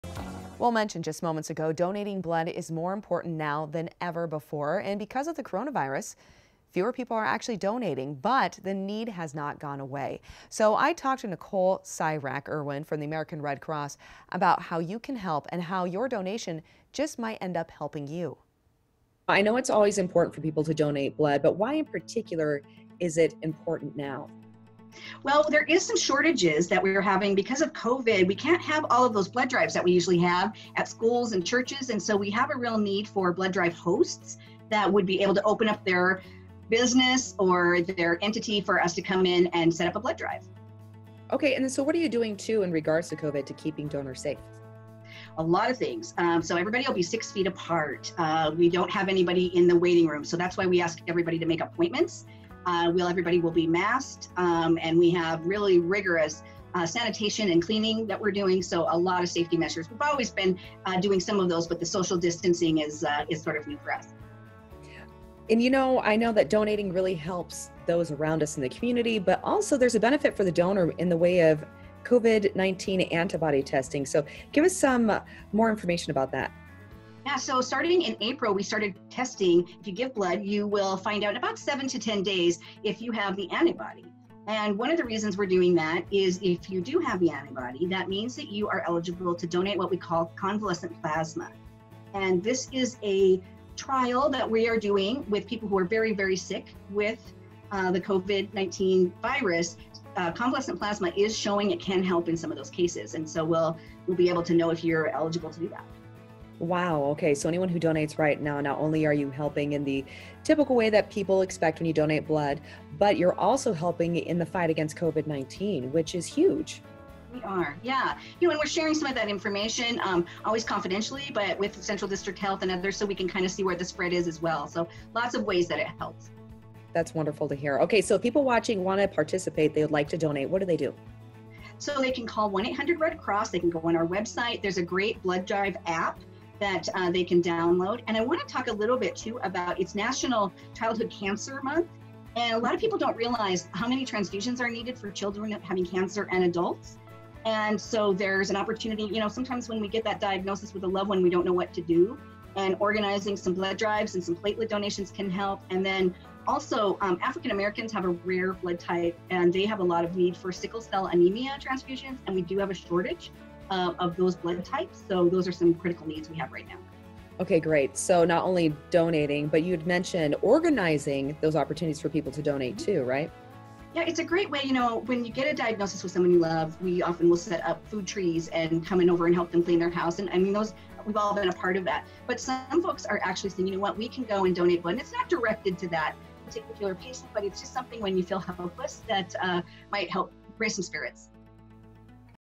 Idaho Today: American Red Cross Blood Donation
Fewer people are donating due to the Coronavirus but the need has not gone down. Nicole with the American Red Cross explains how you can donate safely.